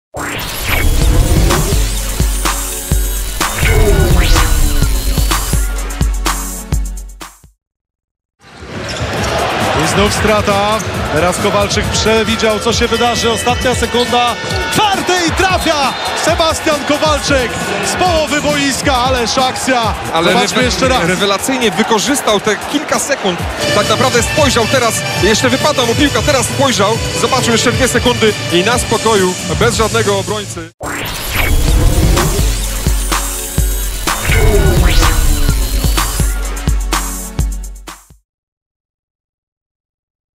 Najbardziej spektakularny rzut sezonu - Sebastian Kowalczyk
Z sezonu na sezon dostaje coraz więcej minut na parkiecie w Asseco. Teraz przede wszystkim występuje w roli strzelca i doskonale uzupełnia się z podstawowym rozgrywającym. Częściej rzuca z dystansu i poprawia swoją skuteczność w tym elemencie. Zadomowiony w Gdyni, gdzie występuje już od czterech sezonów (najpierw w ekipie Startu).

zdjęcia:
montaż: PLK